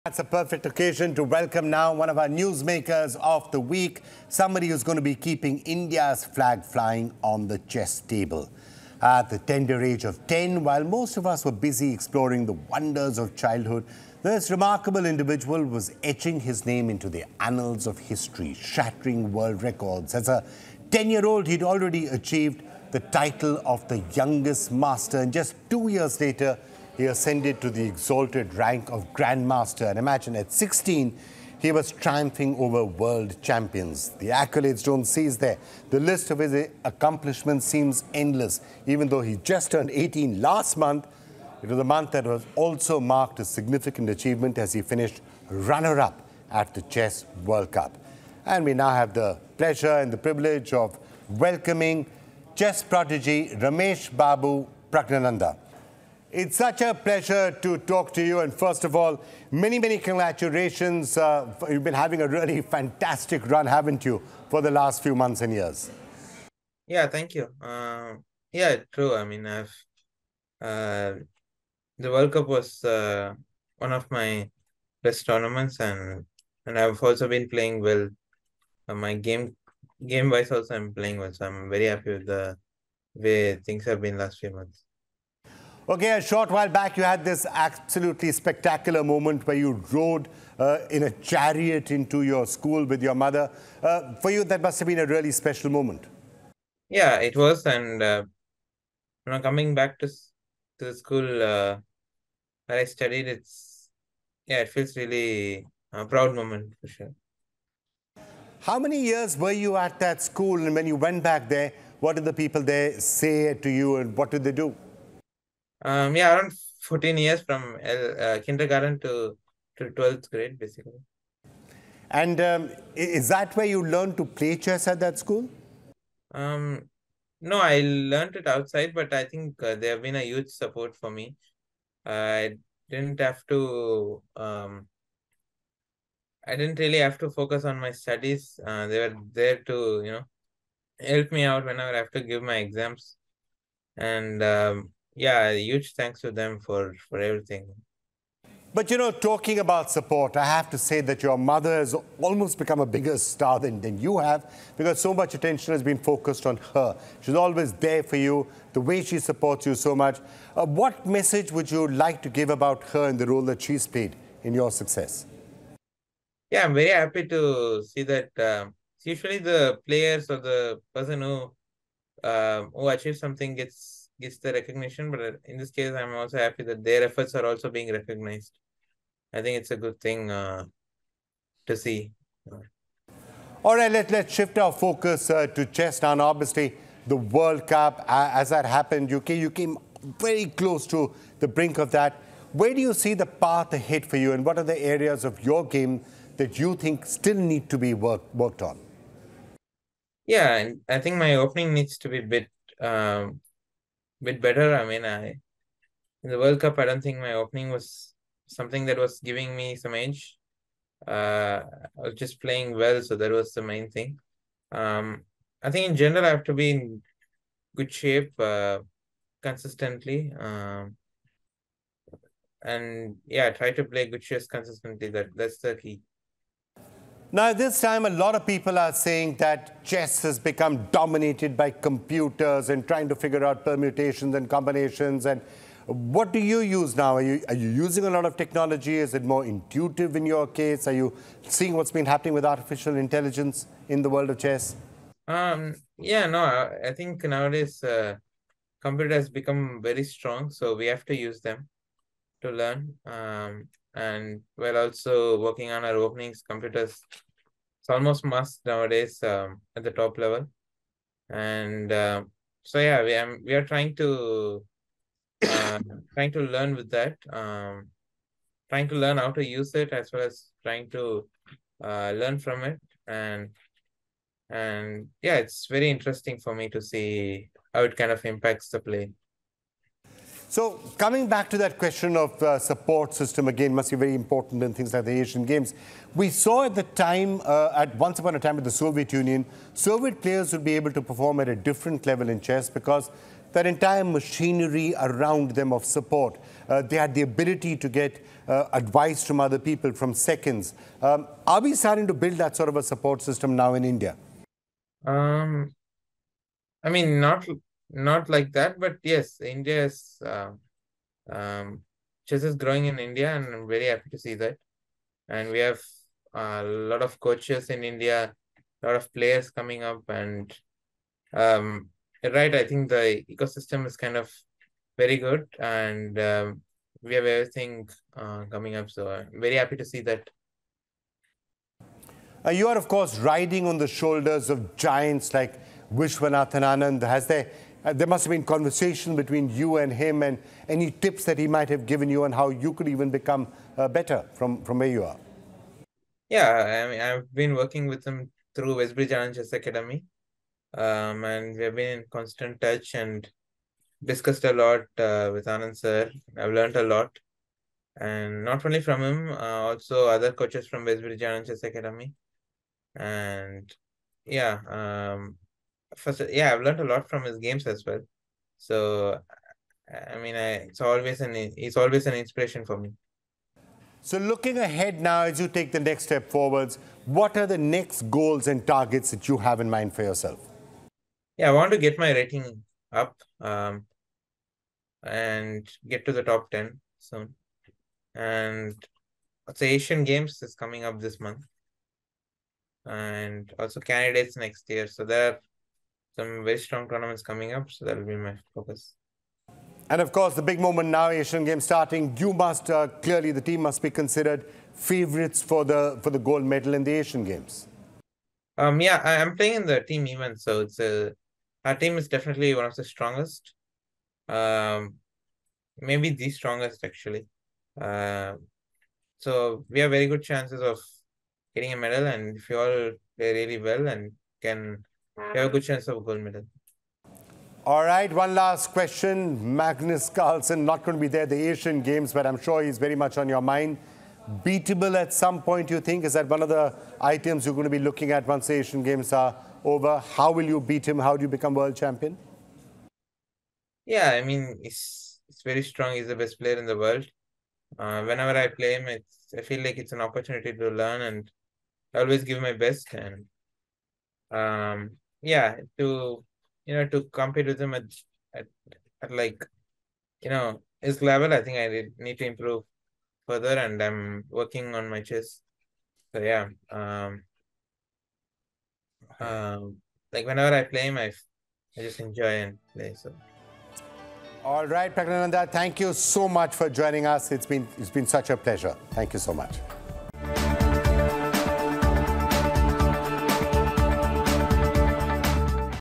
Praggnanandhaa: 'Focused on Asian Games Gold' | The India Story
Watch Vikram Chandra in an exclusive conversation with Grandmaster Rameshbabu ‘Pragg’ Praggnanandhaa on his recent Chess World Cup success and his preparations for the upcoming Asian Games.